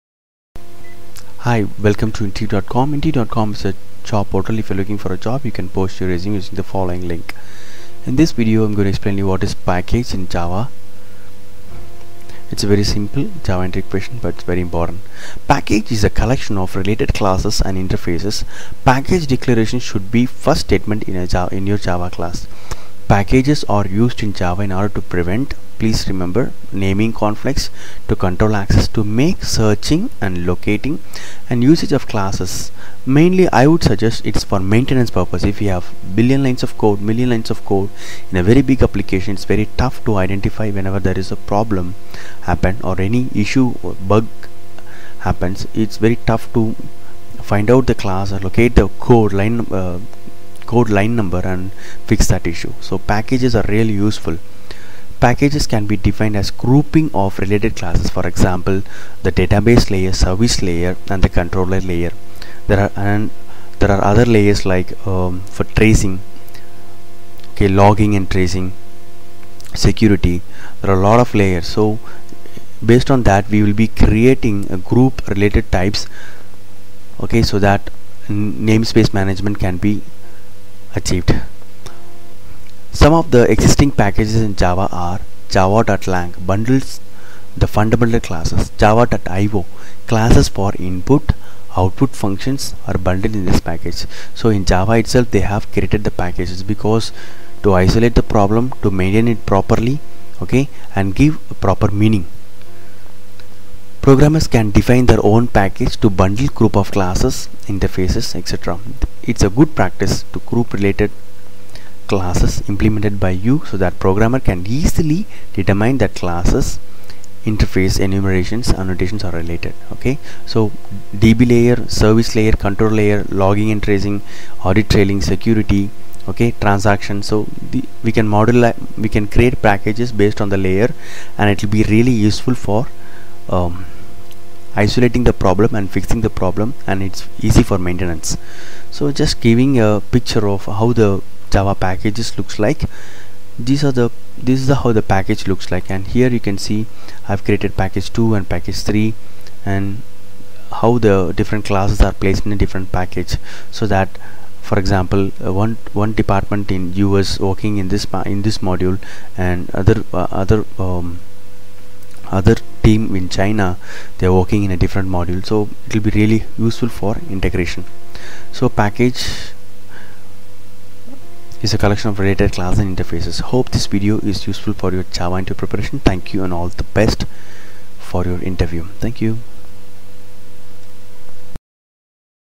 Java Interview Question and Answer What is Java Package Explain
to get notifications. Java Interview Question and Answer What is Java Package Explain
Package is a collection of related classes and interfaces. package declaration should be first statement in a java class.

Packages are used in Java in order to prevent naming conflicts, to control access, to make searching/locating and usage of classes, interfaces, enumerations and annotations easier, etc.

A Package can be defined as a grouping of related types(classes, interfaces, enumerations and annotations ) providing access protection and name space management.

Some of the existing packages in Java are::
 java.lang - bundles the fundamental classes
 java.io - classes for input , output functions are bundled in this package
 Programmers can define their own packages to bundle group of classes/interfaces, etc. It is a good practice to group related classes implemented by you so that a programmer can easily determine that the classes, interfaces, enumerations, annotations are related.